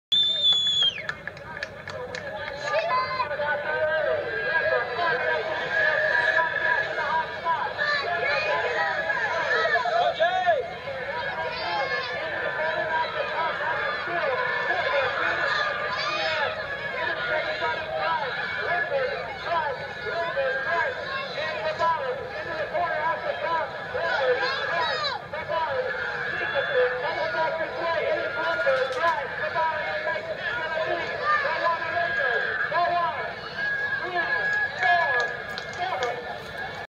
ABA BMX Racing 1989 U.S. Nationals 7 Expert Main
Dustin Lindberg Justin Grise Nick Tabari David Langford Jason Mctygue